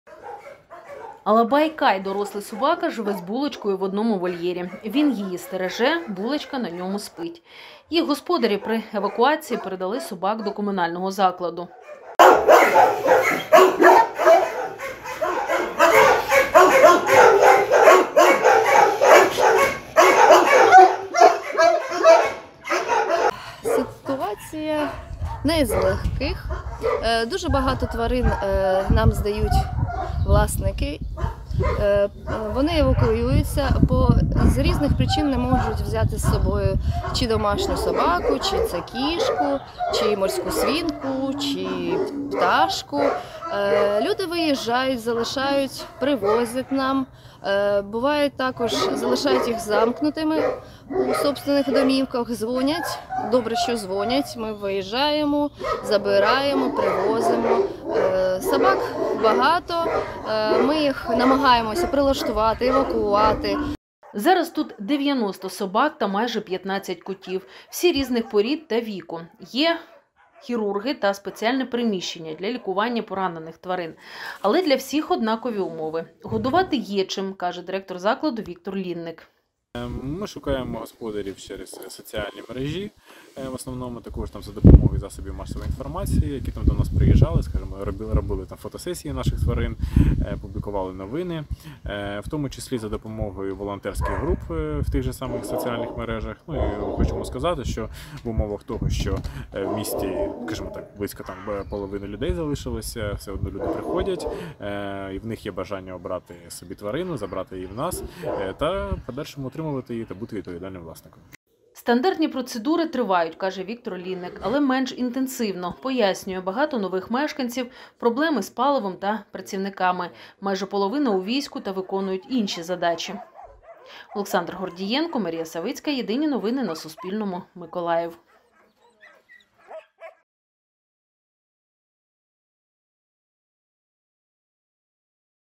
У Миколаєві працює Центр захисту тварин
Покинуті господарями через війну собаки та коти опиняються на вулиці. Про їхню подальшу долю розповідають в міському комунальному підприємстві Центр захисту тварин.
Автори - Олександр Гордієнко, Марія Савицька.